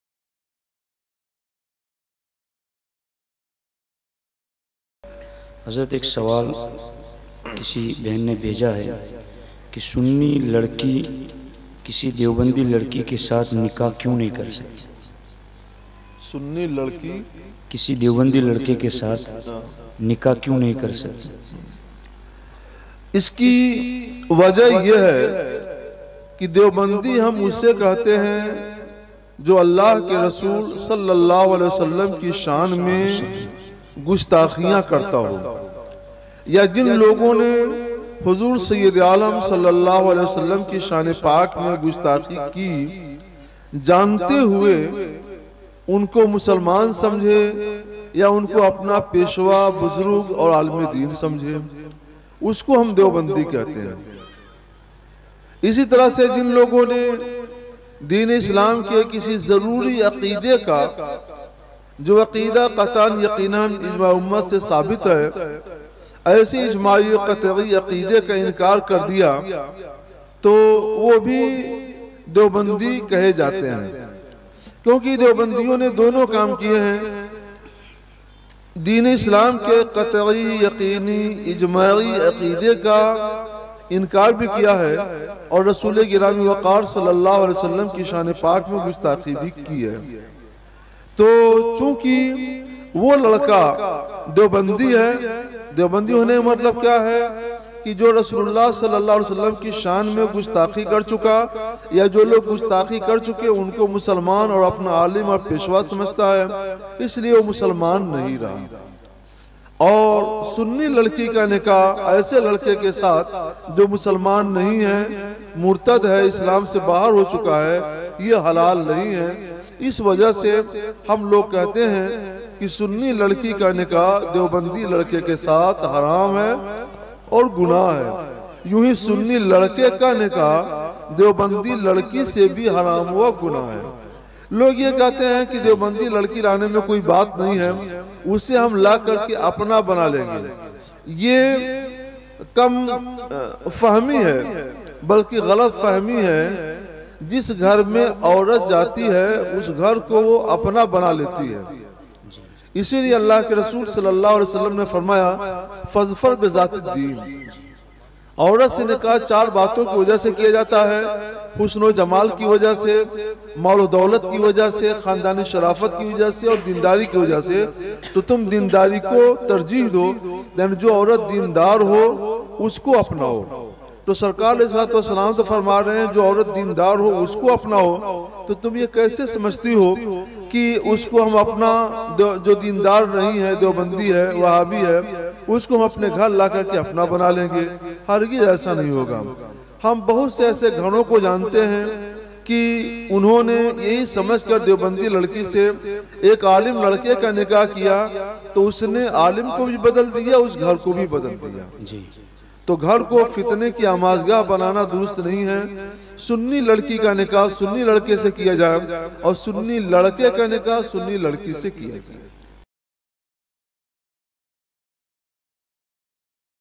Kya Sunni, Deobandi se Nikah Kar Sakta Hai By Mufti Nizamuddin Misbahi
Topic: Kya Sunni Deobandi se Nikah Kar Sakta Hai

Orator:Mufti Nizamuddin Misbahi
(Principal – Al Jamiatul Ashrafiya, Mubarakpur, INDIA)

Day & Date: Saturday 21 November 2015

Place: Bharuch, Gujarat, India

Instagram.com/sdichannel